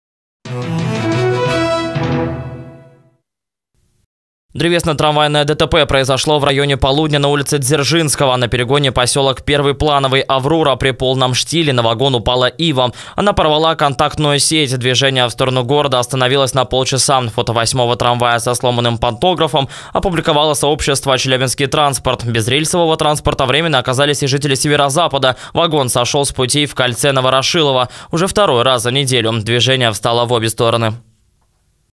Сразу в двух районах города сегодня парализовало движение трамваев
В Ленинском планы пассажиров нарушило дерево, а в Курчатовском - вагон сам сошел с рельсов